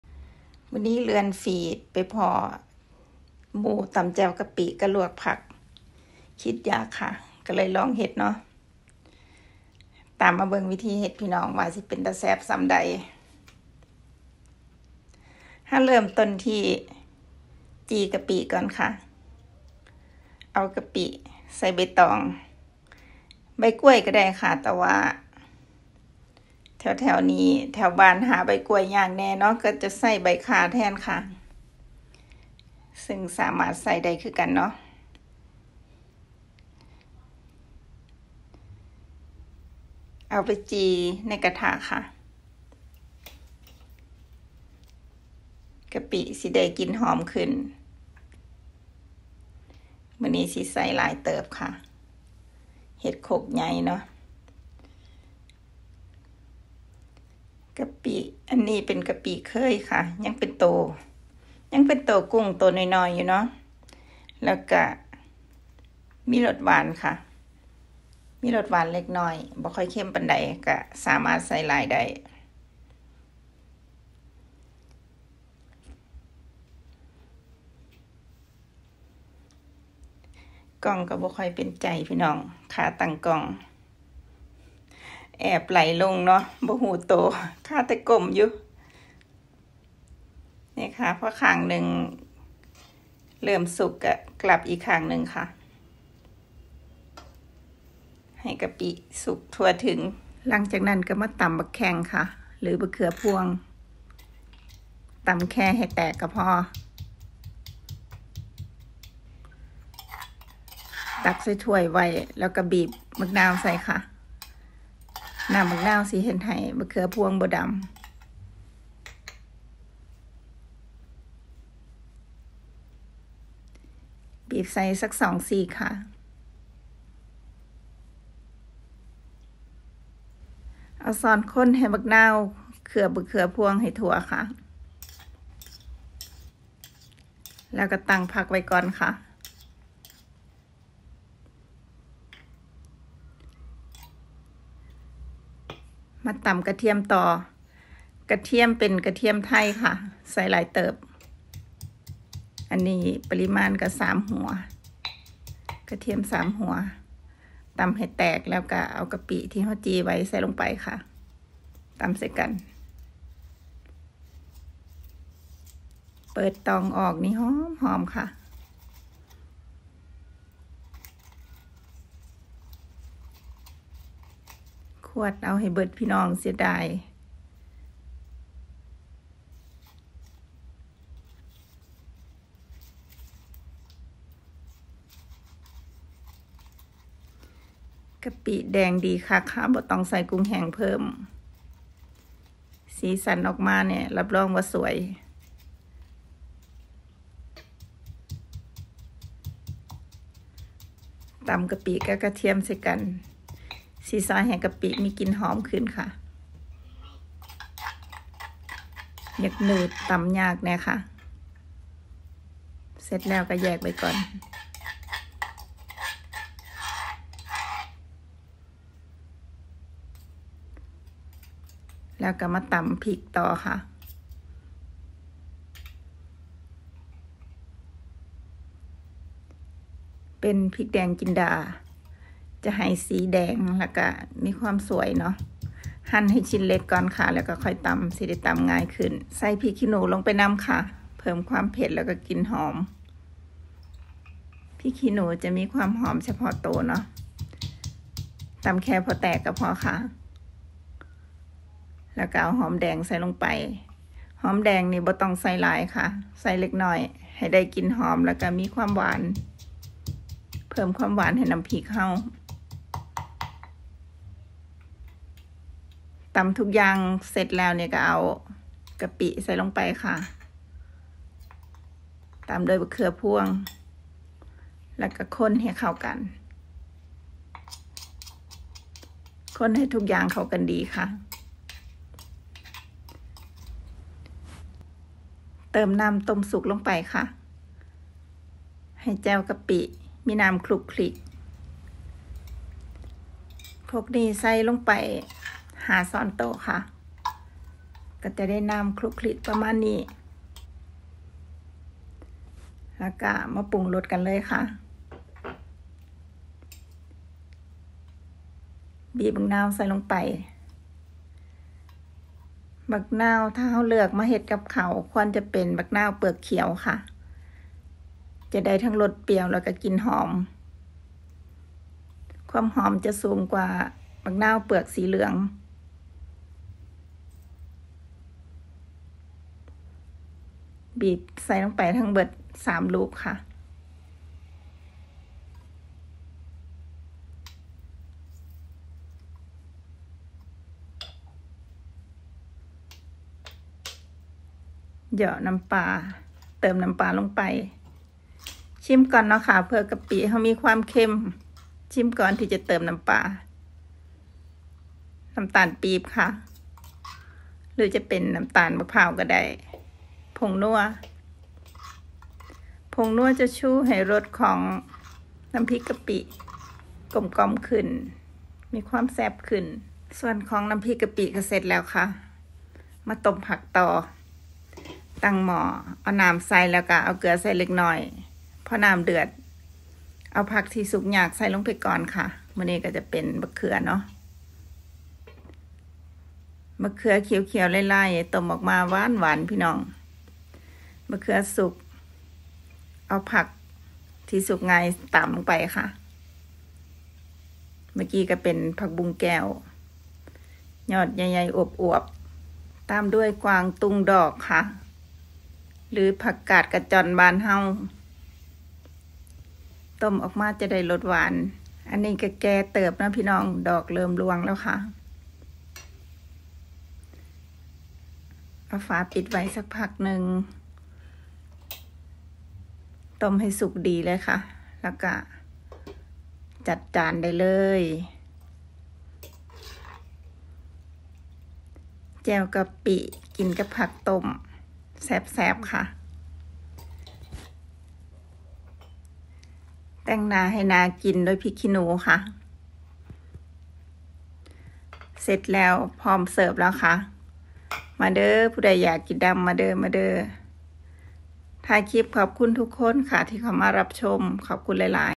น้ำพริกกะปิกับผักต้ม เมนูข้าวหมดหม้อ ตำน้ำพริกกะปิทีไรกินข้าวจานเดียวไม่เคยพอ ￼
น้ำพริกกะปิ เป็นเมนูที่ถูกปากถูกใจหลายๆคน ไม่ว่าจะกินกับผักสดหรือผักต้ม ก็อร่อยเข้ากัน ￼